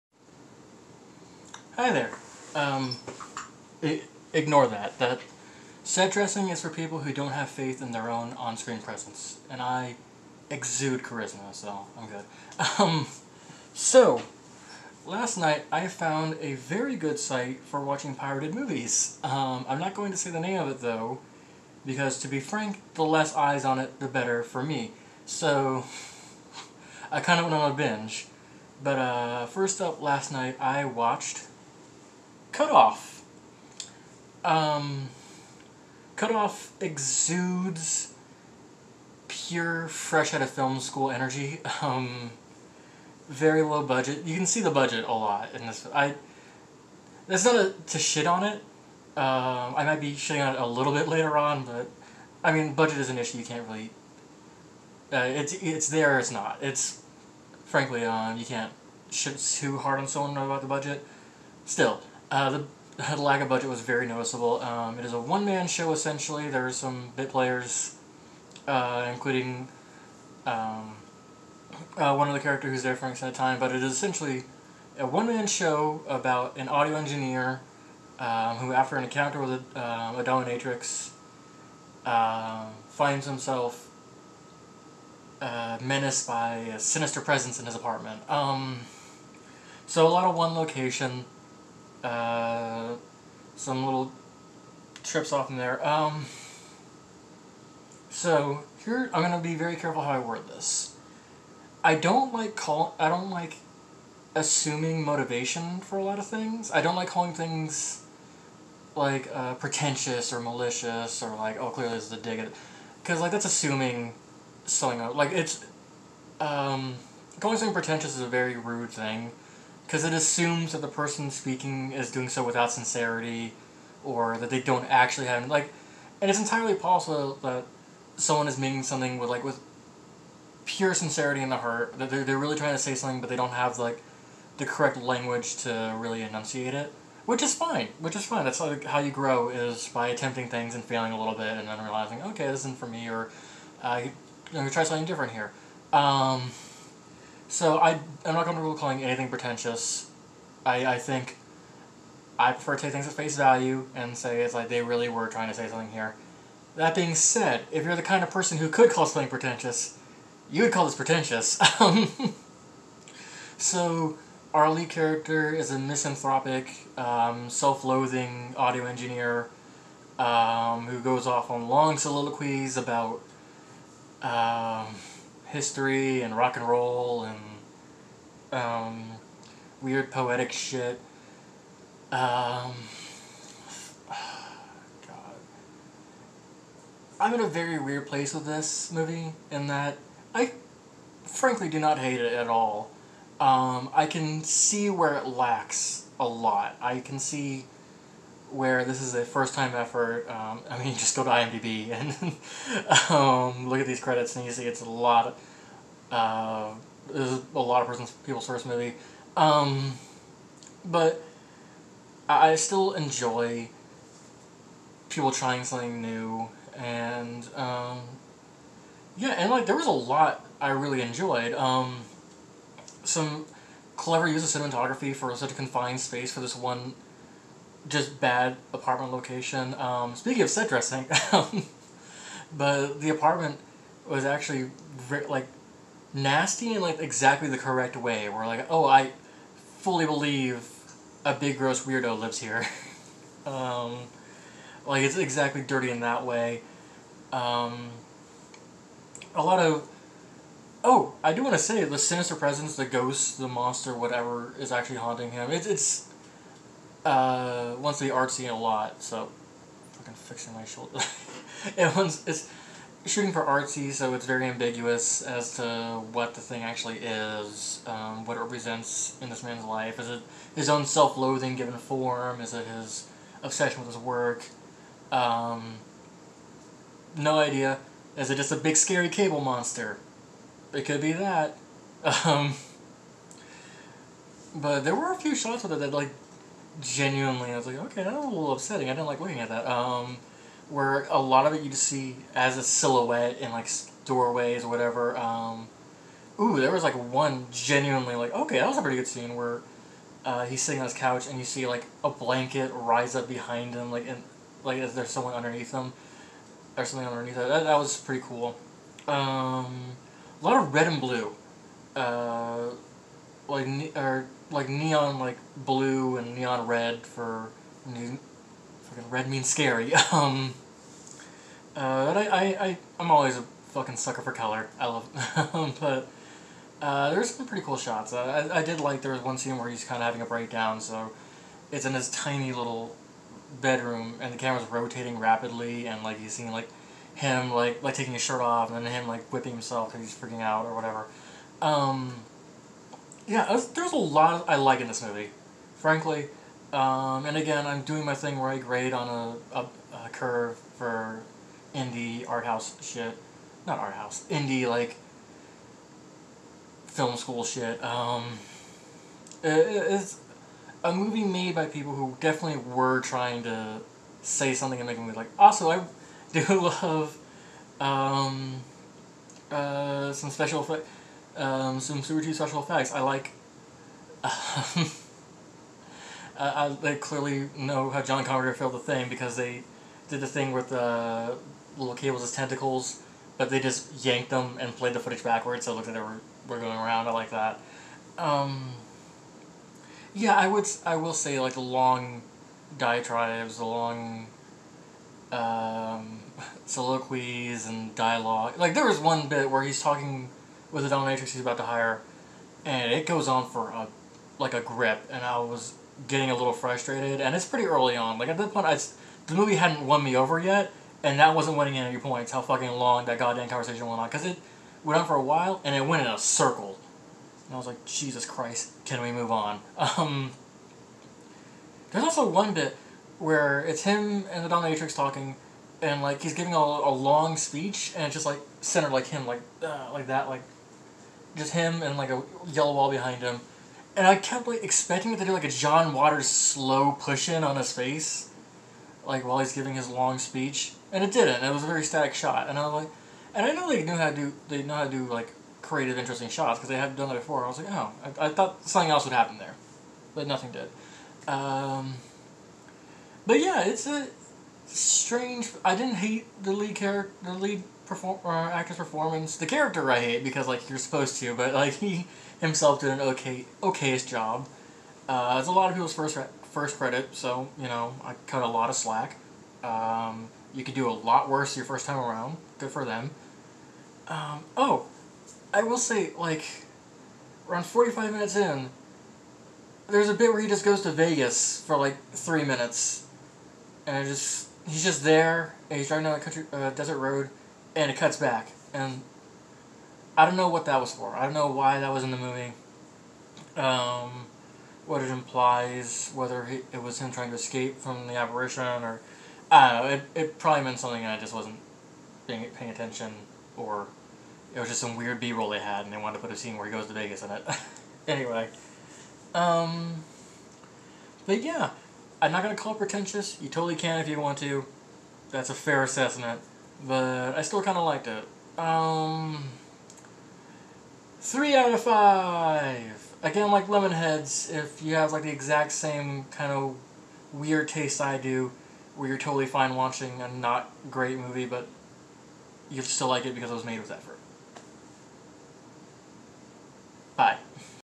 100 Reviews for 100 Movies #61: Cutoff
As one of my many New Year's resolutions this year I'm attempting to watch and give little mini reviews to 100 movies that I haven't see before! Episode 61 is "Cutoff" (2019)

I go out of my way to defend a $5000 indie film that according to iMDB nobody but me liked. But honestly it had some creative cinematography and and some really fun scene compositions. It's a movie clearly made by people who definitely have good ideas and definitely want to make cool weird stuff and I think it should be lauded for that. After all, how are we gonna get cool weird stuff if we don't support the lame weird stuff?

Cutoff isn't currently available on any streaming service, I had to go to a site that probably wasn't on the up and up, so if you wanna watch it rev up that anti-virus.